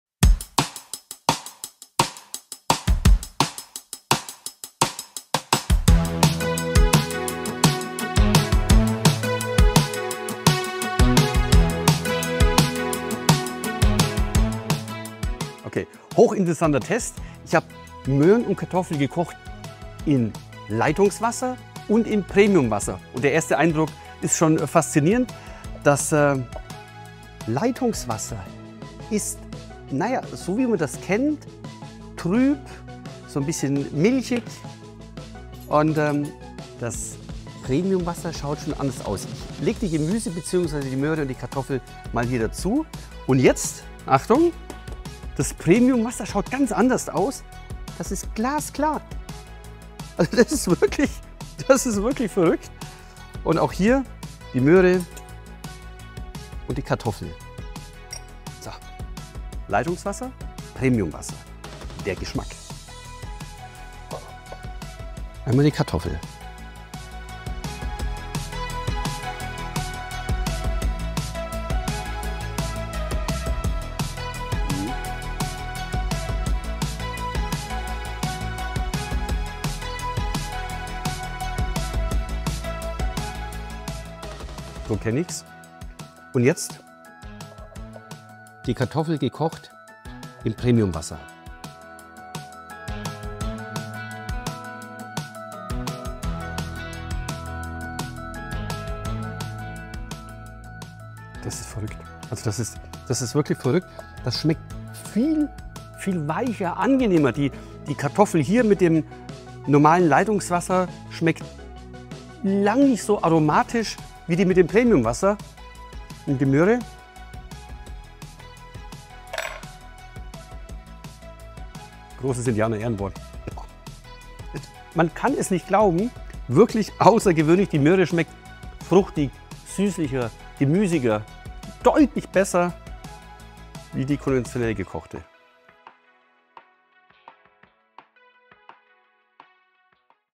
Test delle verdure con lo chef stellato Christian Henze | Water Wolrd | H. Preiss International
Lo chef stellato Christian Henze vive una vera e propria esperienza di gusto – e il motivo è l'acqua premium di H. Preiss International! In questo video mostra come verdure semplici come carote e patate, grazie all'acqua pura, acquisiscano una nuova dimensione di sapore.